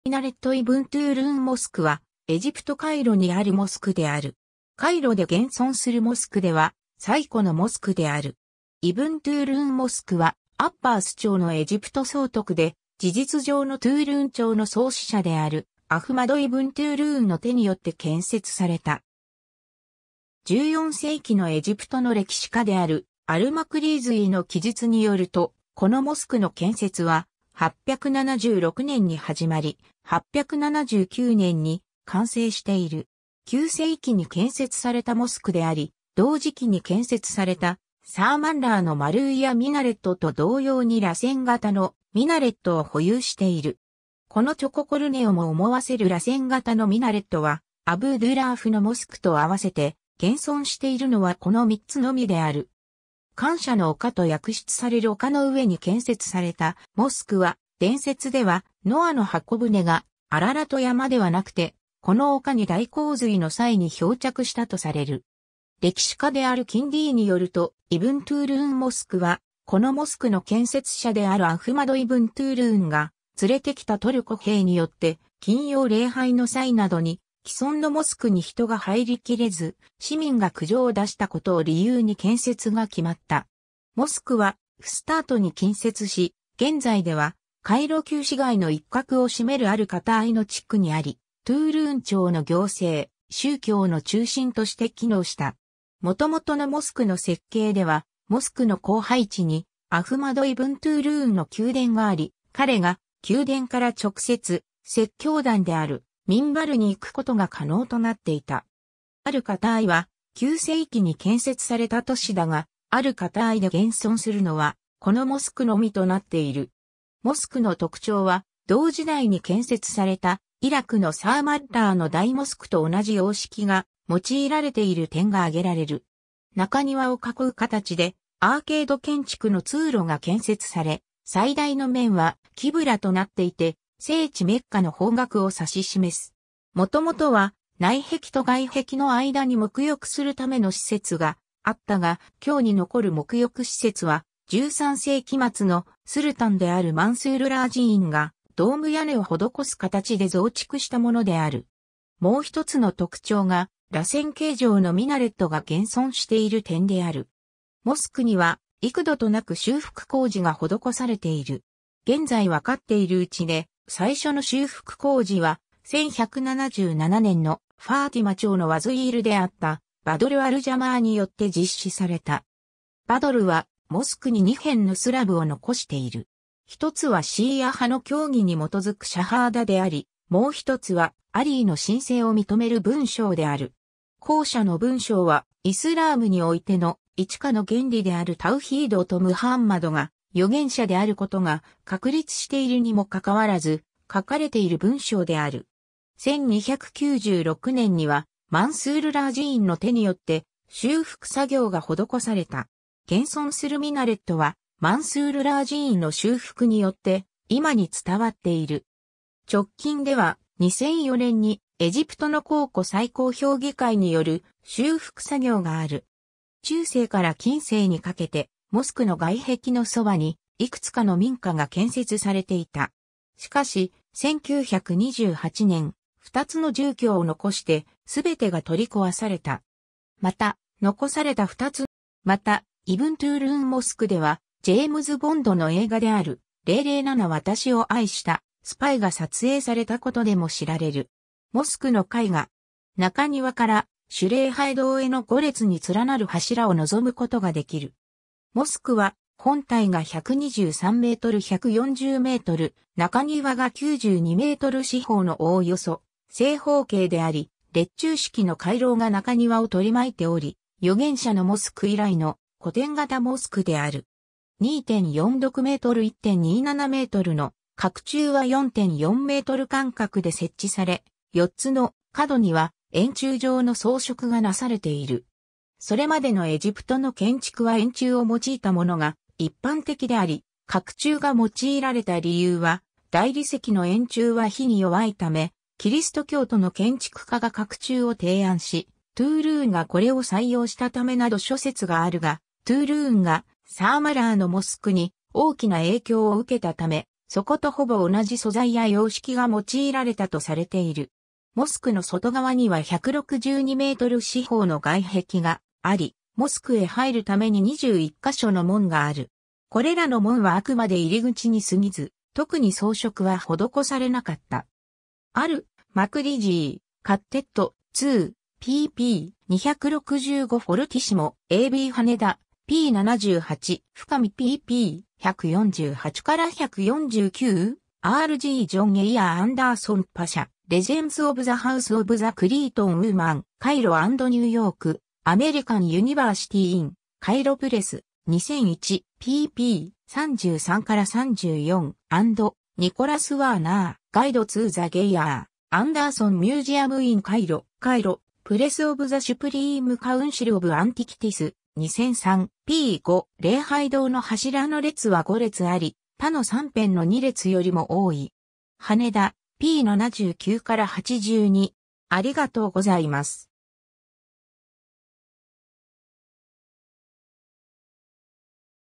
イブン・トゥールーン・モスク
螺旋状のミナレットが残るイブン・トゥールーン・モスク 中庭 ミナレット イブン・トゥールーン・モスク（アラビア語:مسجد أحمد بن طولون）は、エジプト・カイロにあるモスクである。
カイロで現存するモスクでは最古のモスクである。
 イブン・トゥールーン・モスクは、アッバース朝のエジプト総督で、事実上のトゥールーン朝の創始者であるアフマド・イブン・トゥールーンの手によって建設された。
14世紀のエジプトの歴史家であるアル＝マクリーズィーの記述によると、このモスクの建設は、876年に始まり、879年に完成している。
 9世紀に建設されたモスクであり、同時期に建設されたサーマッラーのマルウィヤ・ミナレットと同様に螺旋型のミナレットを保有している。
このチョココルネをも思わせる螺旋型のミナレットは、アブー・ドゥーラーフのモスクとあわせて、現存しているのはこの3つのみである。
 「感謝の丘」と訳出される丘の上に建設されたモスクは、伝説では、ノアの方舟がアララト山ではなくて、この丘に大洪水の際に漂着したとされる。
 歴史家であるキンディーによると、イブン・トゥールーン・モスクは、このモスクの建設者であるアフマド・イブン・トゥールーンが連れてきたトルコ兵によって金曜礼拝の際などに既存のモスクに人が入り切れず、市民が苦情を出したことを理由に建設が決まった。
モスクは、フスタートに近接し、現在ではカイロ旧市街の一角を占めるアル・カターイの地区にあり、トゥールーン朝の行政、宗教の中心として機能した。
もともとのモスクの設計では、モスクの後背地にアフマド・イブン・トゥールーンの宮殿があり、彼が宮殿から直接、説教壇であるミンバルに行くことが可能となっていた。
アル・カターイは、9世紀に建設された都市だが、アル・カターイで現存するのは、このモスクのみとなっている。
 モスクの特徴は、同時代に建設されたイラクのサーマッラーの大モスクと同じ様式が用いられている点が挙げられる。
中庭を囲う形で、アーケード建築の通路が建設され、最大の面は、キブラとなっていて、聖地メッカの方角を指し示す。
もともとは、内壁と外壁の間に沐浴するための施設があったが、今日に残る沐浴施設は、13世紀末のスルタンであるマンスール・ラージーンがドーム屋根を施す形で増築したものである。
もう1つの特徴が、螺旋形状のミナレットが現存している点である。
 モスクには幾度となく修復工事が施されている。
現在分かっているうちで、最初の修復工事は、1177年のファーティマ朝のワズィール（宰相）であったバドル・アル＝ジャマーリーによって実施された。
バドルは、モスクに2編のスラブを残している。
1つはシーア派の教義に基づくシャハーダ（信仰告白）であり、もう1つは、アリーの神性を認める文章である。
後者の文章は、イスラームにおいての「一化」の原理であるタウヒードとムハンマドが預言者であることが、確立しているにもかかわらず、書かれている文章である。
 1296年には、マンスール・ラージーンの手によって、修復作業が施された。
現存するミナレットは、マンスール・ラージーンの修復によって、今に伝わっている。
直近では、2004年に、エジプトの考古最高評議会による修復作業がある。
 中世から近世にかけて、モスクの外壁のそばに、いくつかの民家が建設されていた。
しかし、1928年、2つの住居を残して、すべてが取り壊された。
また、残された2つの住居はモスクの近隣の住居では最古のものの2つであり、よい状態で保存されている。
 また、イブン＝トゥールーン・モスクでは、ジェームズ・ボンドの映画である『007_私を愛したスパイ』が撮影されたことでも知られる。
 モスクの絵画。
中庭から主礼拝堂への5列に連なる柱を望むことが出来る。
 モスクは、本体が123m×140m、中庭が92m四方のおおよそ正方形であり、列柱式の回廊が中庭を取り巻いており、預言者のモスク以来の「古典型」モスクである。
2.46m×1.27mの角柱は4.4m間隔で設置され、4つの角には円柱状の装飾がなされている。
それまでのエジプトの建築は円柱を用いたものが一般的であり、角柱が用いられた理由は「大理石の円柱は火に弱いため」「キリスト教徒の建築家が角柱を提案し、トゥールーンがこれを採用したため」など諸説があるが、トゥールーンがサーマッラーのモスクに大きな影響を受けたため、そことほぼ同じ素材や様式が用...